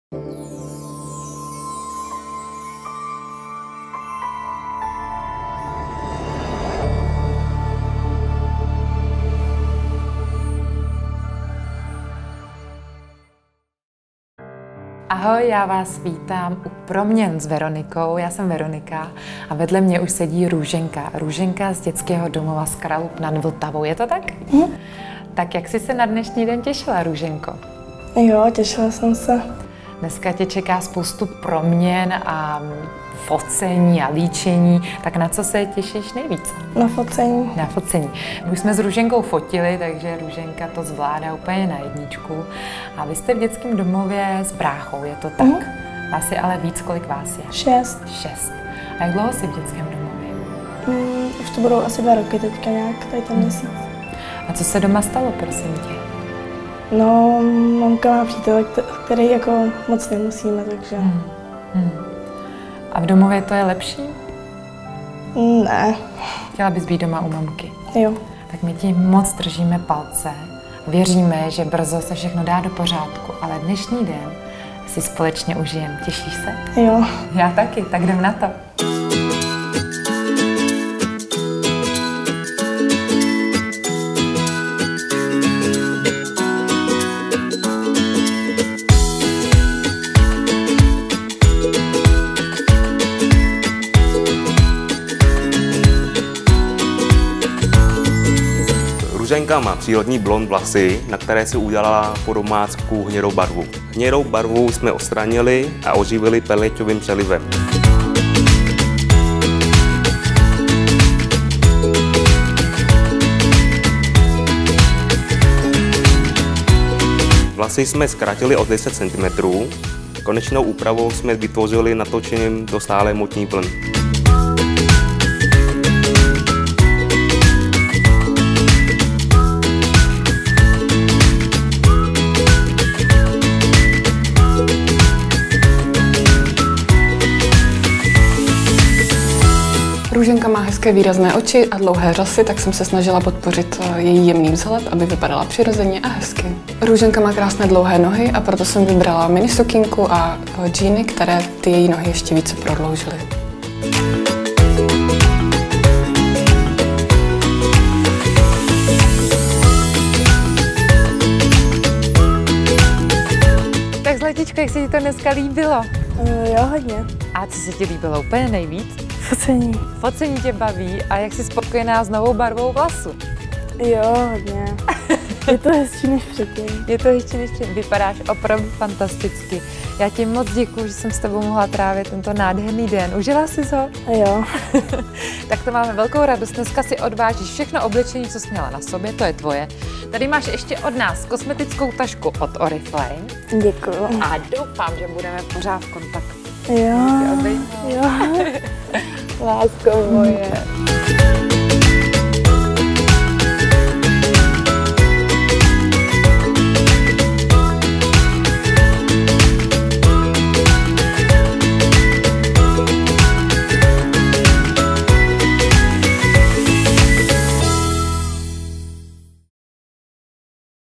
Proměny s Veronikou - Růženka
První letošní proměna patří naší krásné Růžence. Růženka má ještě pět sourozenců. A společně s bráškou teď žijí v DD Kralupy nad Vltavou. Protože si nerozumí s novým přítelem jejich maminky. Budeme jim držet palečky, aby se všechno vyřešilo, tak jak si přejí.
Růženka byla krásná blondýnka, ale bohužel se rozhodla, že by byla ráda brunetka. Vlásky si sama obarvila na hnědou. Lukáš je, ale kouzelník a dokázal jí její nádhernou blond vrátit. My moc děkujeme.  A doufáme, že si  i Růženka uvědomila, že takhle je nejkrásnější.
Pojďte se na její proměnu podívat společně s námi.

Velice děkujeme všem partnerům projektu za podporu, zvláště pak:  Oriflame, Profimovie.com, H&M, Václav Pražák HAIR Design, Ateliér Rafael a Natálie Host.
NFVK.